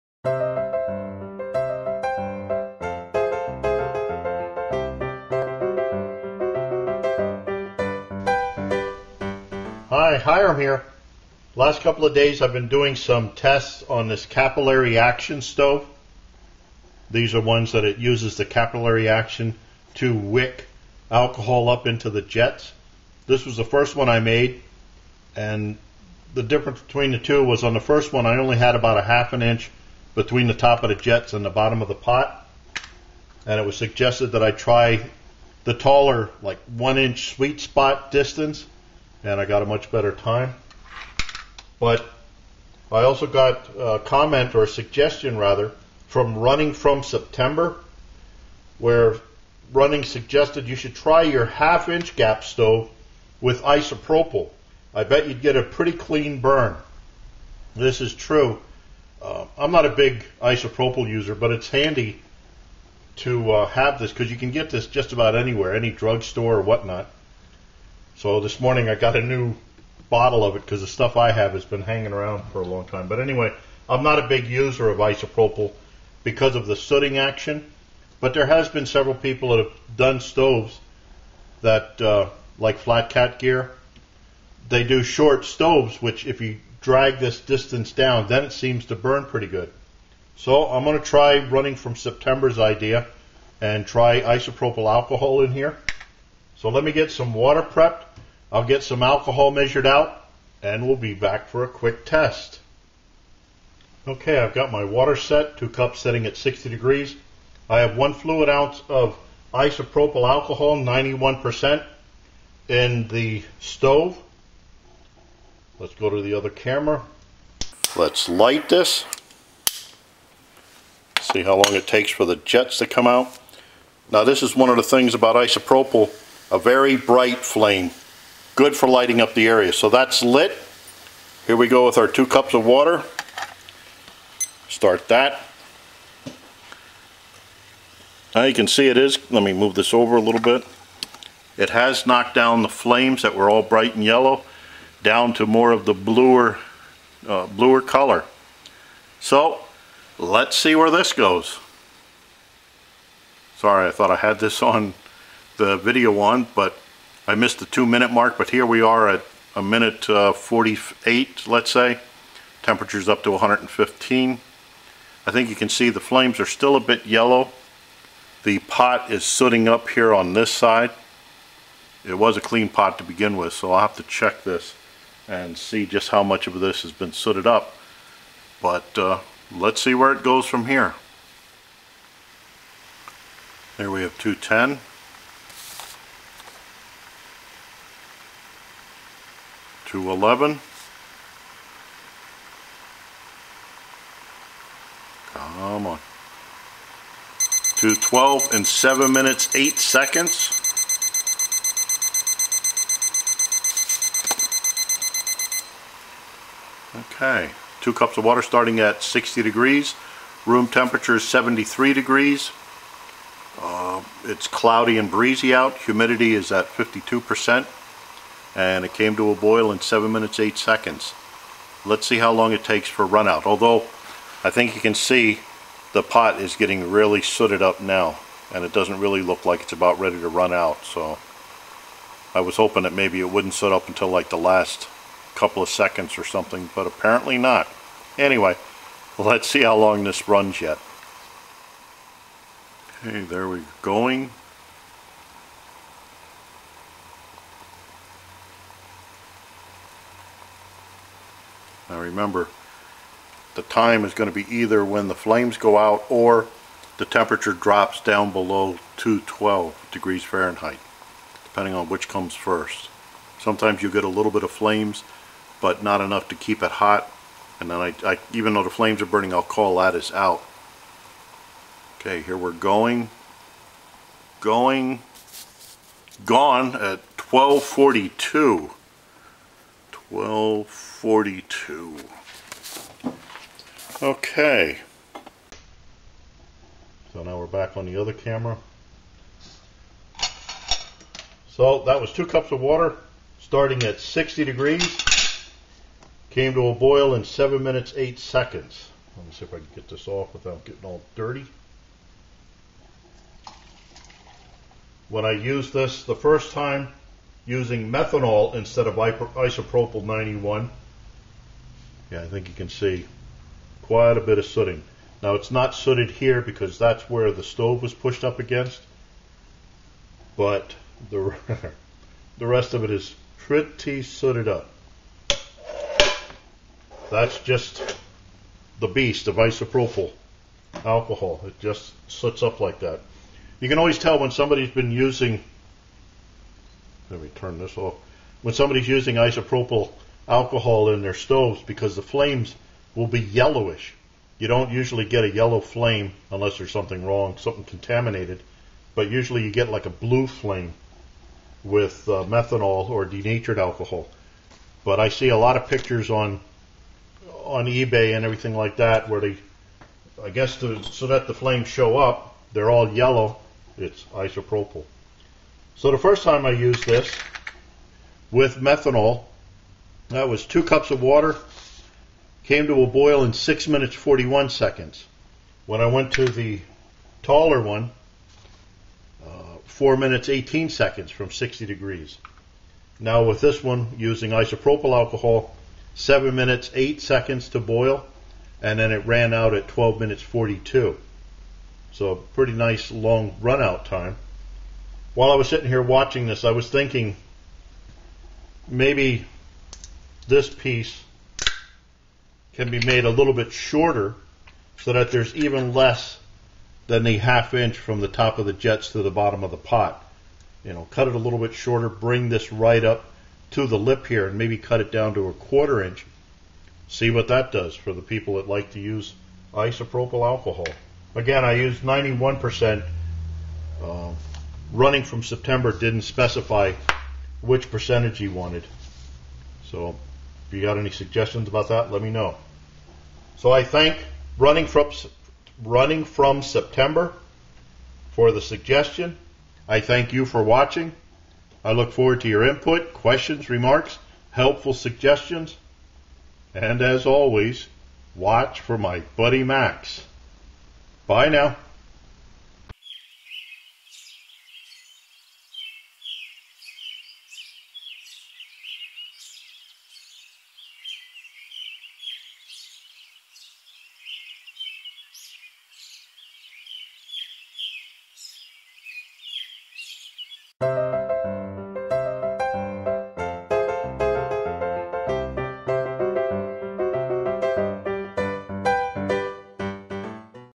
Capillary Action Stove (CAS#1) - Isopropyl Alcohol - Boil Test #1
I've been doing a couple of boil tests with Capillary Action Stoves that I've made. In this video I'm trying an idea that runningfromseptember suggested, "you should try your 1/2 inch gap stove with isopropyl. i bet you would get a pretty clean burn."

I'm not a big Isopropyl alcohol user because I don't like all the soot it generates but if it works in the short Capillary Action Stove this could very handy for the people that do like Isopropyl alcohol. Plus it would be handy to be able to use alcohol that you stockpile in case we get into one of those SHTF situations.

I thank runningfromseptember for the suggestion,
Hiram

Lab0492
Title: Capillary Action Stove (CAS#1) - Isopropyl Alcohol - Boil Test #1
Room temp: 73°F (22.7°C)
Humidity: 61%
Weather condition: Sunny - Clear
Burner: Capillary Action Stove (CAS) #1
Stove/Pot stand: Capillary Action Stove (CAS) #1
Pot: Boy Scout Pot #2
Fuel: Isopropyl Alcohol 91%
Amount of fuel: 1.0 fluid ounces (30 ml)
Amount of water: 2 cup(s) = 0.47 liter
Water temp at start: 60°F (15.5°C)
Time to boil: 7 minute(s) 08 second(s)
Time to run-out: 12 minute(s) 42 second(s)
Temperature at run-out: NA
Note:
NA = Time to boil with CAS#1 with methanol: 6 minute(s) 41 second(s)

Specs on the Capillary Action Stove (CAS#1):
Diameter: 2.25" (57.3 mm)
Height(of stove): 1.60" (40.6 mm)
Height(of stove - bottom): 1.0" (25.5 mm)
Height(of stove - top): 1.38" (35.1 mm)
Distance from jets to pot bottom: 1/2"
Weight (empty): .23 ounce (6.5 grams)
Fuel Capacity: 1 oz (30 ml) +
Made of: aluminum soda can - 7.5 fl.oz (222ml) size